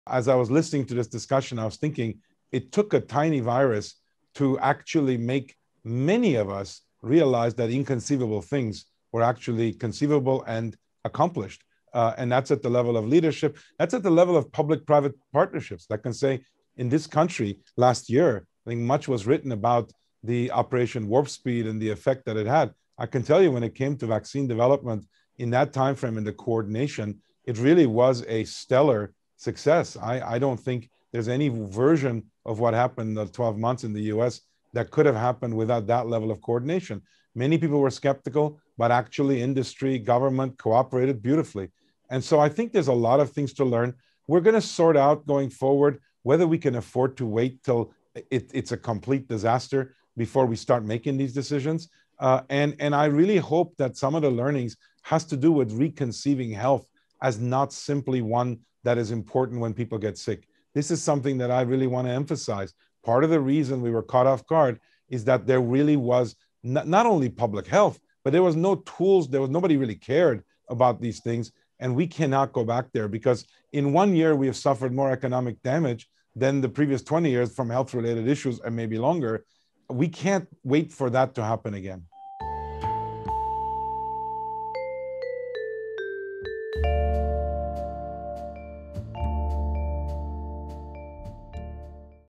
How a Tiny Virus Helped Accomplish the ‘Inconceivable’ | Moderna's Noubar Afeyan | GZERO Media
“It took a tiny virus for many of us to realize that inconceivable things were actually conceivable.”

“It took a tiny virus for many of us to realize that inconceivable things were actually conceivable” on cooperation between the US government and the private sector on COVID vaccine development, says Moderna co-founder Noubar Afeyan. Afeyan, also CEO of the Flagship Pioneering venture capital fund, credits Operation Warp Speed as a “stellar success” without which America would not have recovered from COVID as fast as it has.